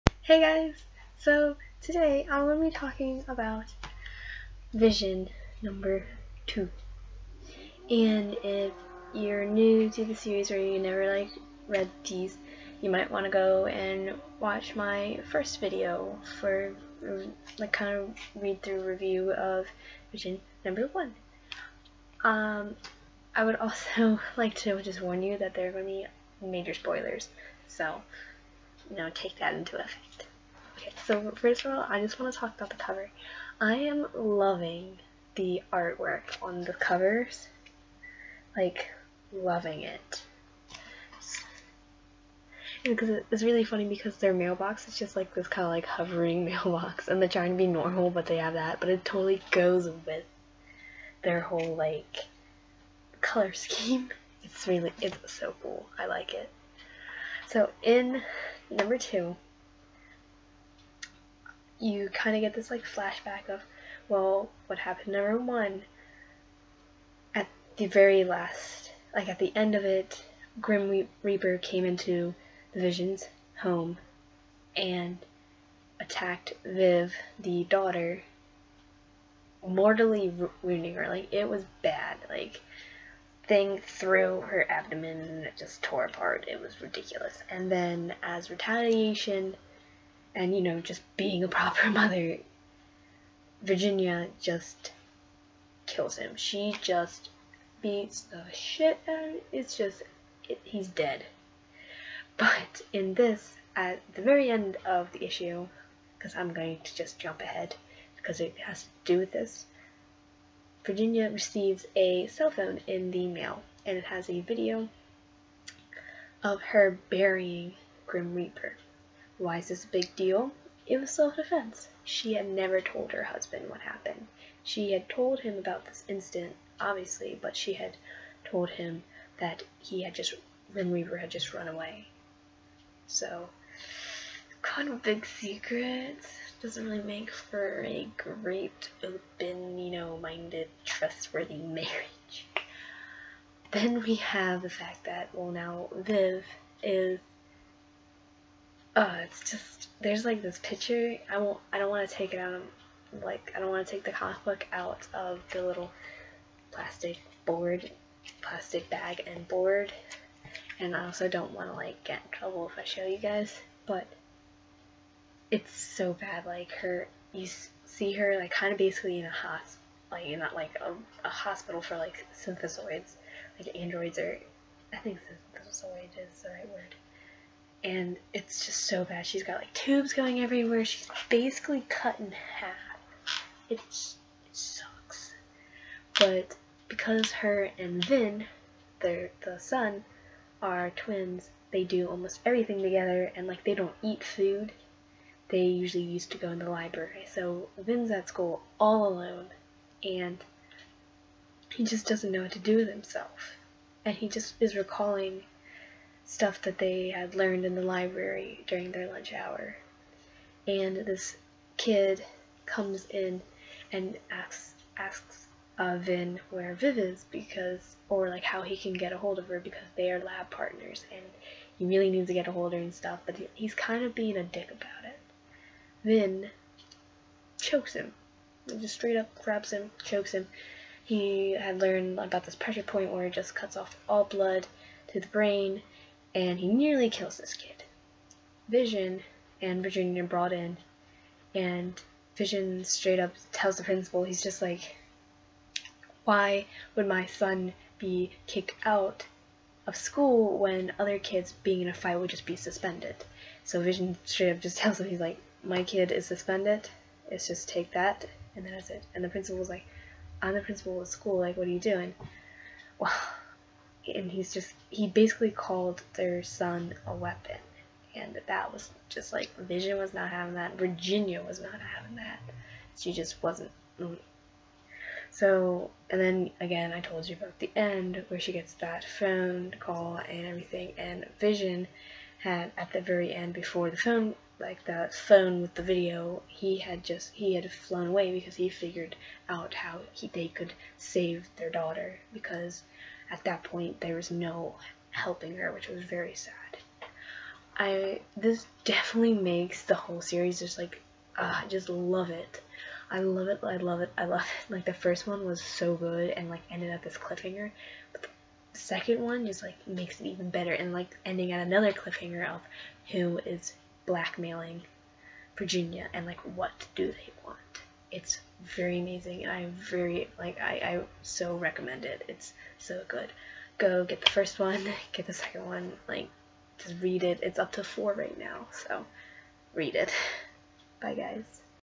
Reading The Vision #2
A great second issue for this series. I highly recommend it for anyone. In the comments let me know what your reading and what you think I should read.

If you are feeling generous, I would appreciate any help with my trip to Ireland.